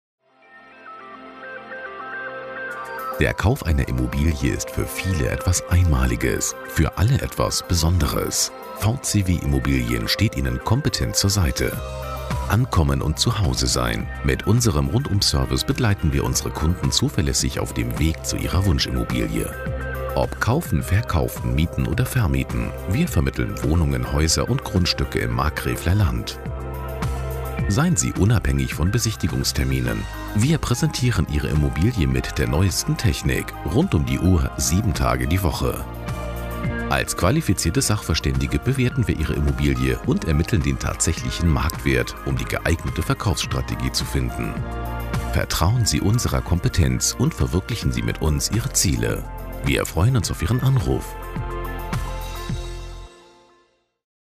vcw-immo - Ihr Immobilien- und Sachverständigenbüro in Bad Krozingen
Der Kauf einer Immobilie ist für viele etwas Einmaliges im Leben, für alle etwas Besonderes. Für diesen Schritt sollte der Kunde Zuverlässigkeit und kompetente Begleitung erwarten können. Immobilienmaklerin Vicky Wilhelm steht Ihnen zur Seite.

vcw-immo
Vicky Wilhelm
Hartheim am Rhein / Bad Krozingen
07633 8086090